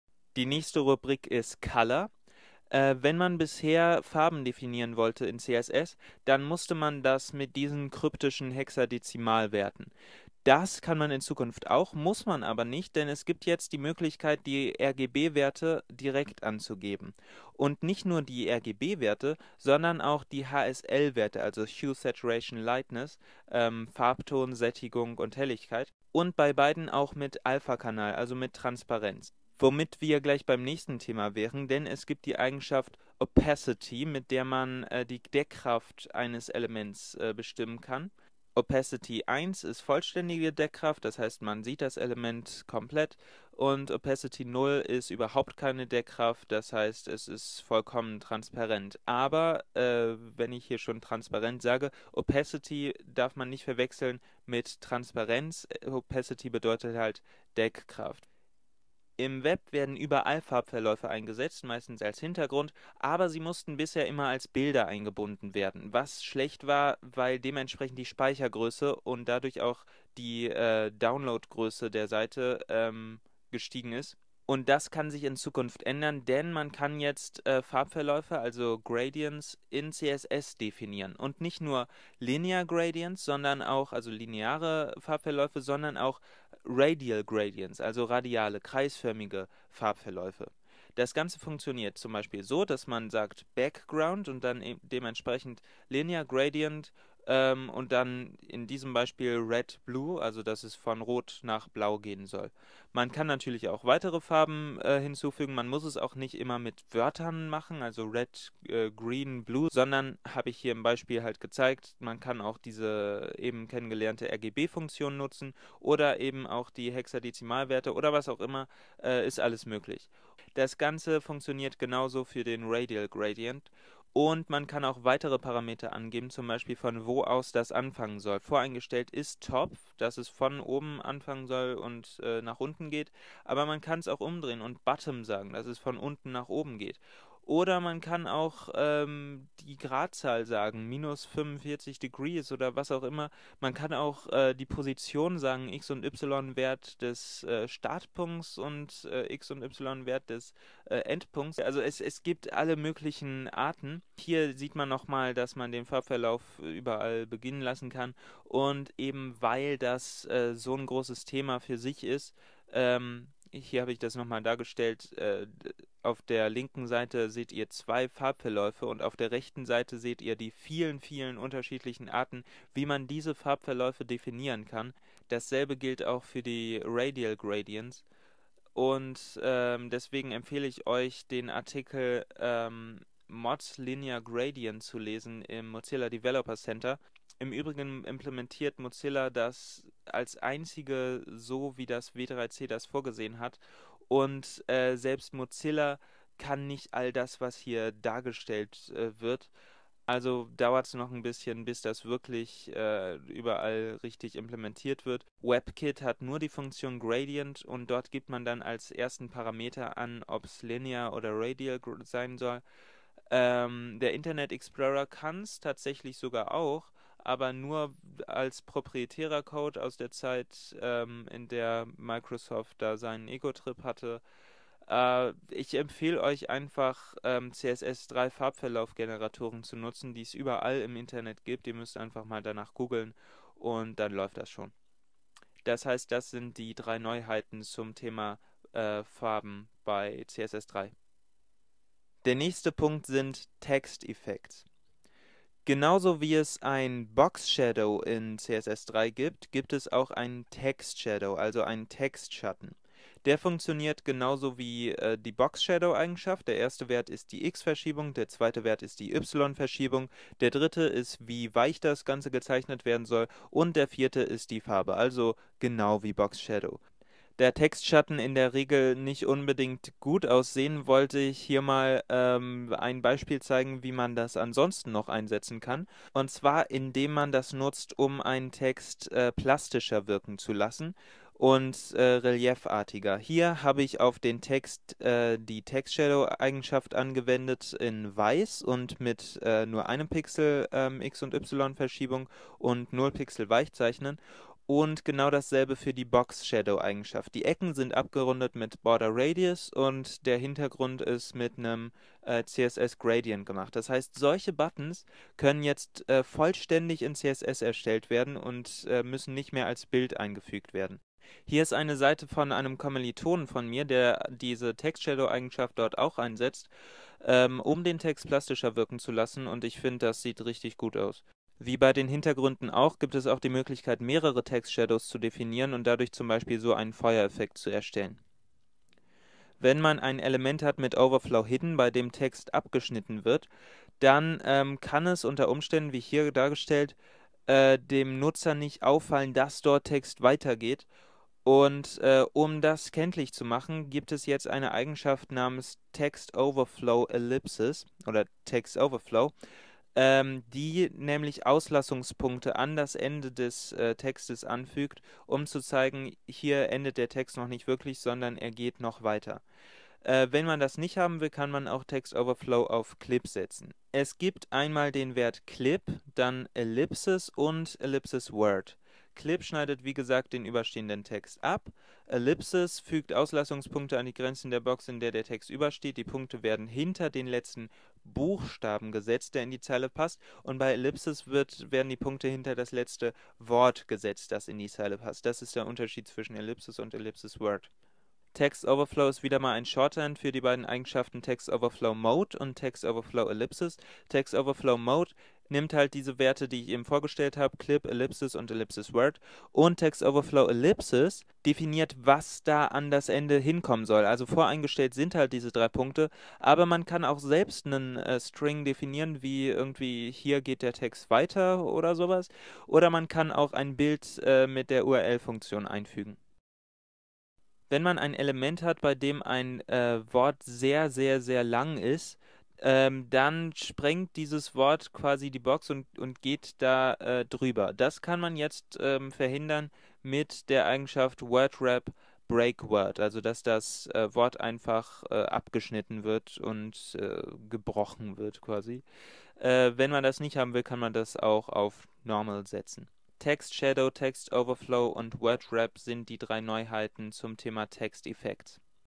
CSS3 Eigenschaften #2 (Color, Text Effects)
Die CSS3 Eigenschaften zu den Themen
Color: rgb(), hsl(), rgba(), hsla(), opacity, linear-gradient, radial-gradient
und Text Effects: text-shadow, text-overflow, word-wrap